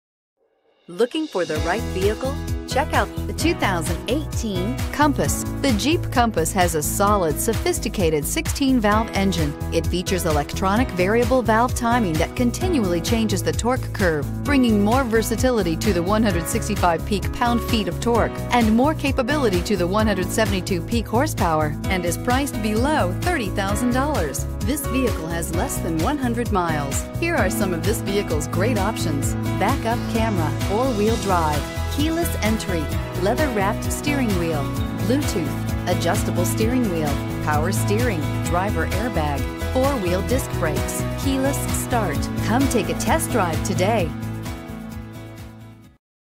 2018 Jeep Compass JT283434
2018 JEEP COMPASS Paramus, NJ
Stock# JT283434

Delivers 30 Highway MPG and 22 City MPG! This Jeep Compass delivers a Regular Unleaded I-4 2.4 L/144 engine powering this Automatic transmission. WHEELS: 17  X 7.0  POLISHED/GRANITE POCKETS ALUM, TRANSMISSION: 9-SPEED 9HP48 AUTOMATIC, RADIO: UCONNECT 4 W/7  DISPLAY.

* This Jeep Compass Features the Following Options *
QUICK ORDER PACKAGE 27J , POWER 8-WAY DRIVER/MANUAL 6-WAY PASSENGER SEATS, POPULAR EQUIPMENT GROUP, MONOTONE PAINT APPLICATION, MANUFACTURER'S STATEMENT OF ORIGIN, JAZZ BLUE PEARLCOAT, ENGINE: 2.4L I4 ZERO EVAP M-AIR W/ESS, COMPACT SPARE TIRE, COLD WEATHER GROUP, BLACK, PREMIUM CLOTH/VINYL BUCKET SEATS.

A short visit to Dodge Chrysler Jeep Ram Of Paramus located at 315 Rt 4 West, Paramus, NJ 07652 can get you a trustworthy Compass today!